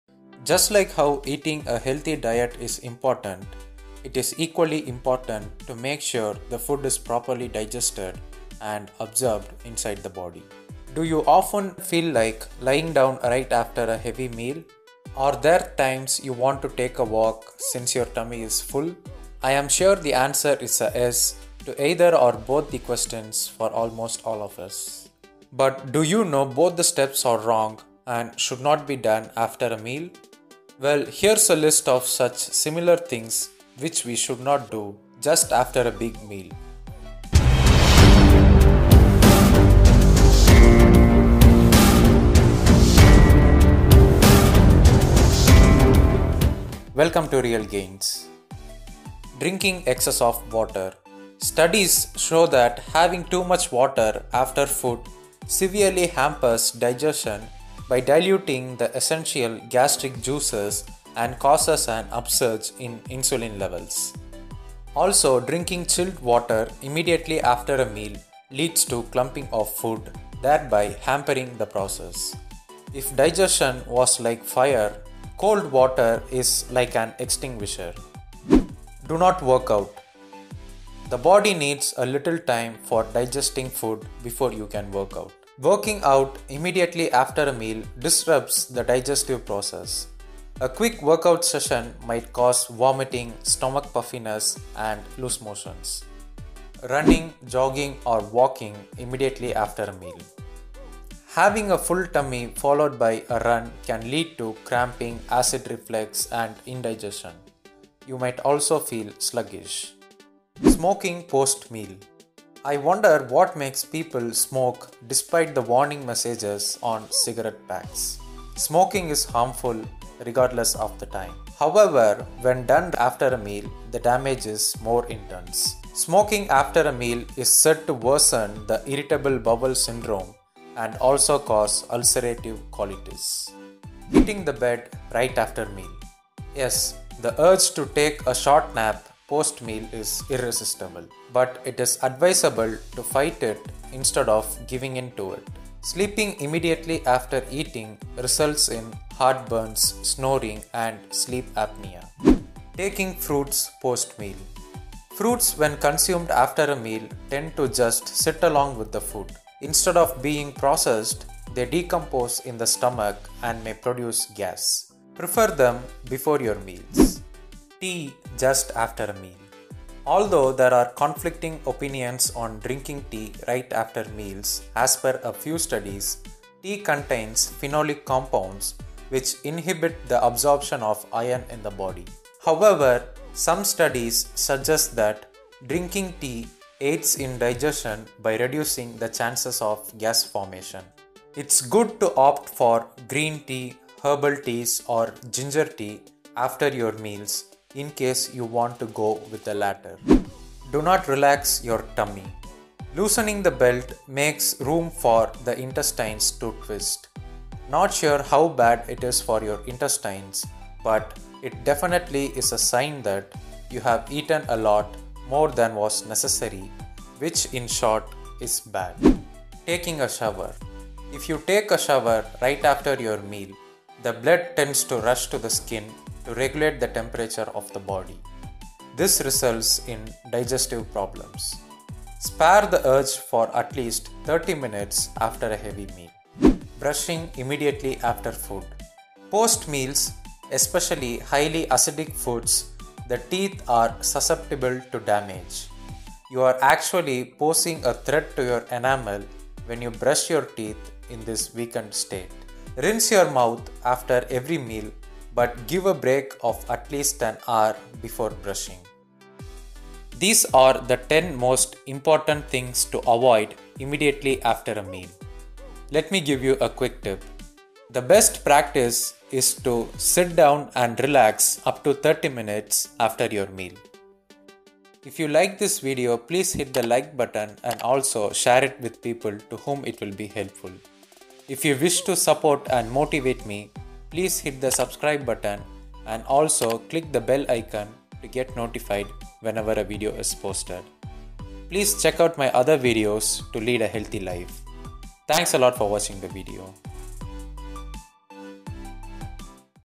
10 Things To Avoid After A Meal
10 Things To Avoid After A Meal

Just like how eating a healthy diet is important it is equally important to make sure the nutrients are absorbed in to the body effectively.
That's why it is important to avoid the things that can hamper digestion immediately after a big meal.